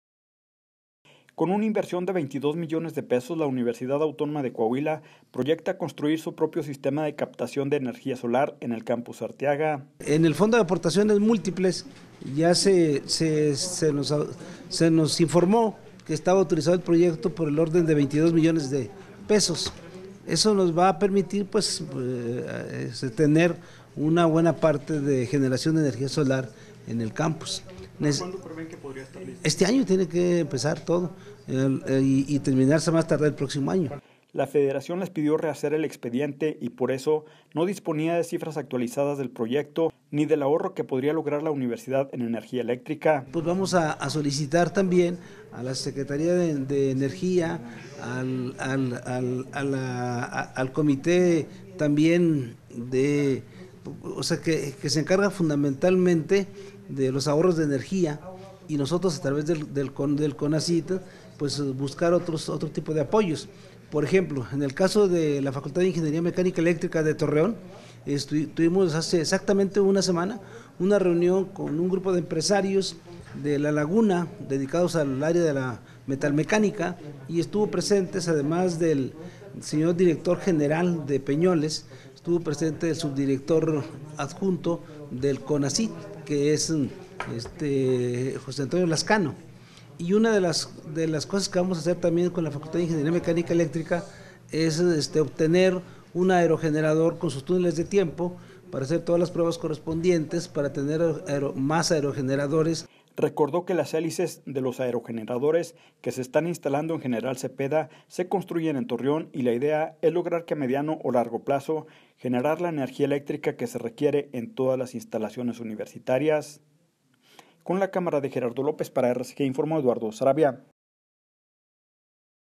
Proyecta UAdeC instalar colectores solares de energía
Con una inversión de 22 millones de pesos la Universidad Autónoma de Coahuila proyecta construir su propio sistema de captación de energía solar en el campus Arteaga.